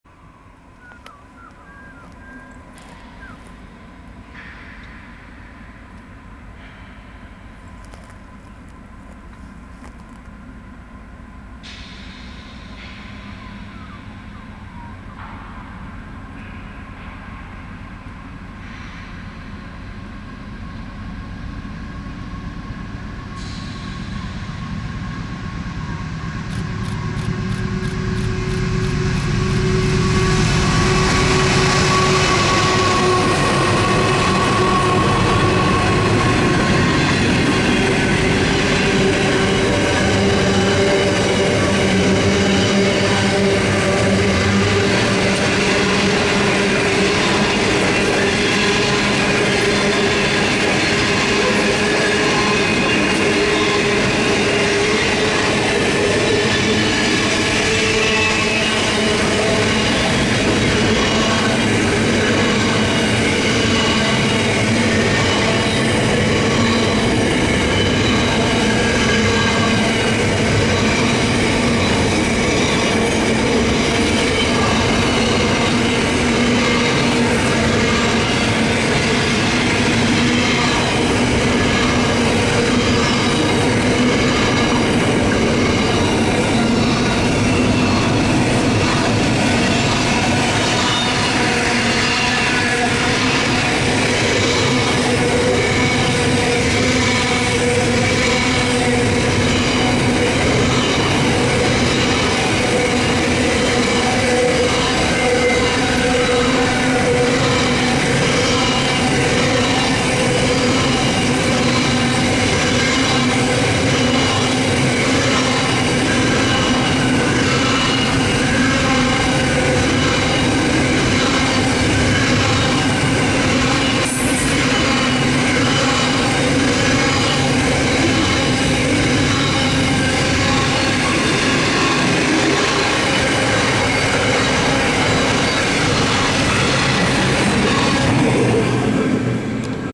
NR95 and NR98 on 5PM5 freight Parkeston
NR95 and NR98 on 5PM5 freighter seen here heading though the outskirts of Parkeston for Adelaide/Melbourne on the 13th August 2021